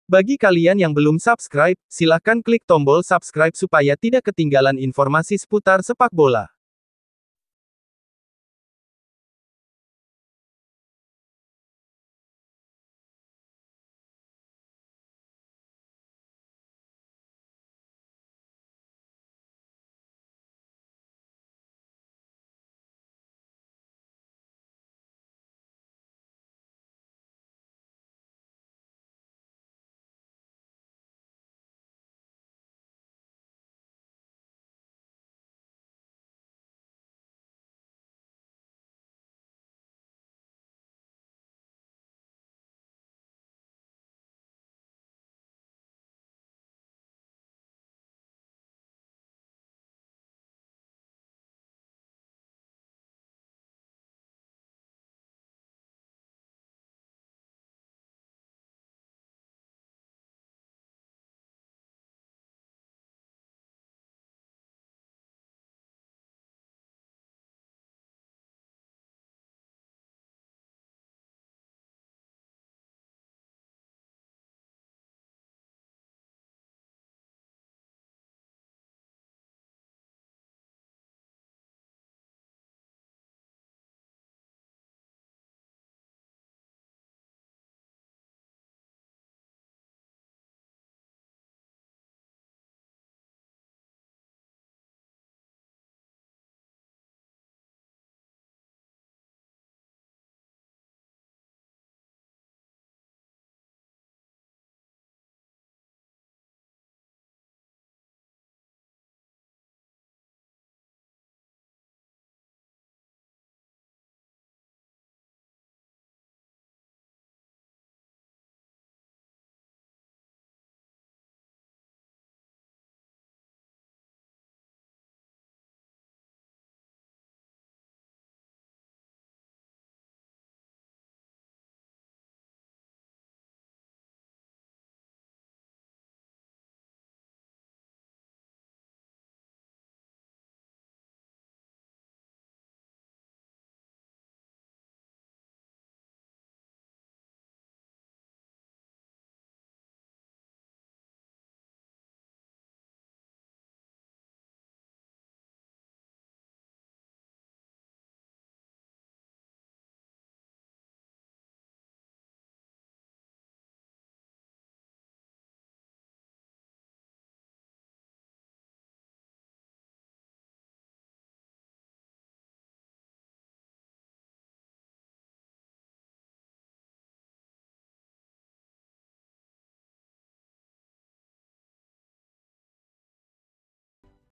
Transfer pemain Persiraja putaran kedua liga 1 2021
Persiraja kabarnya sudah mulai melakukan
negosiasi dengan beberapa pemain dan pelatih untuk putaran kedua liga 1 2021.

diantara:
1.  BRUNO DYBAL
2.  RIJAL "TORRES"
3.  BRIAN FERREIRA
4.  SERGIO ALEXANDRE